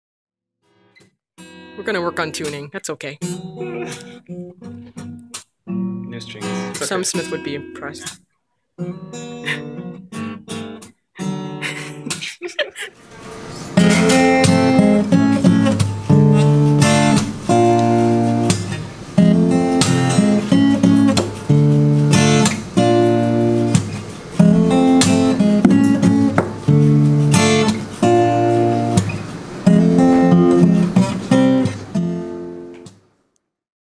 String Replacement: Before and After
We installed a new set of Elixir strings on Miguel's acoustic Squier guitar.  In the "Before" footage, the new strings are still settling (clearly); in the "After" footage the strings are finally staying in tune.